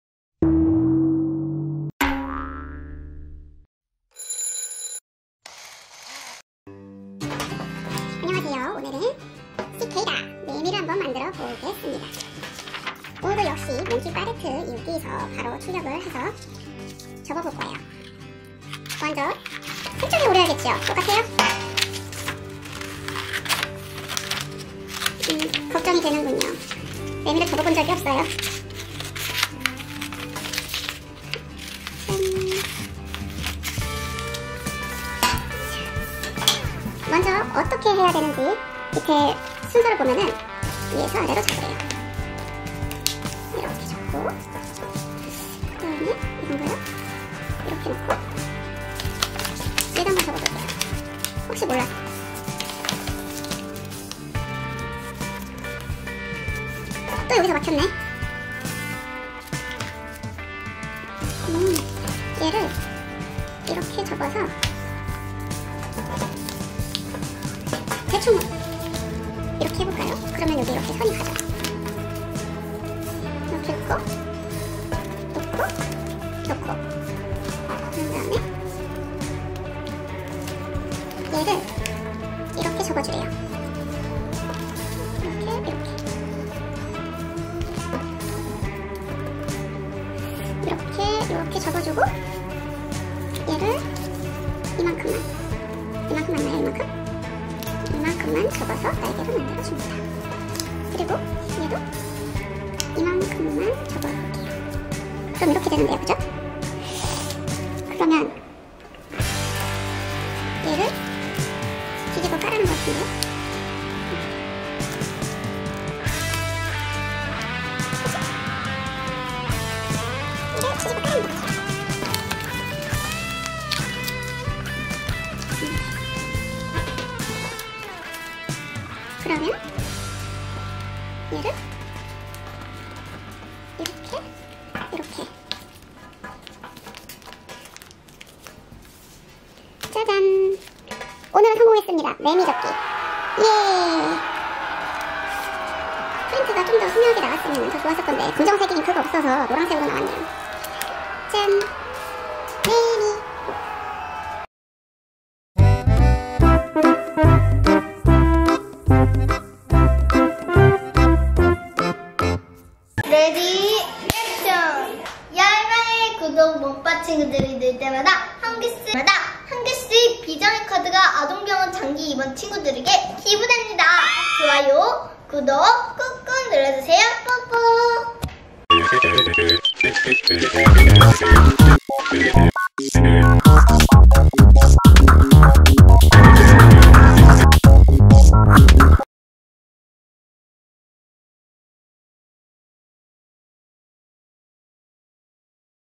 엉터리 종이접기2탄] 매미 접기
코로나로 집에만 있는 아이들에게 상상하고 놀 수 있는 자유를! 학습효과는 덤~

이런 색종이는 첨이야~~~
집에서 직접 출력해서 접어 볼 수 있도록 무료소스 제공하고 있답니다.
몽키빠렛트 홈페이지 자료실에서 만나요!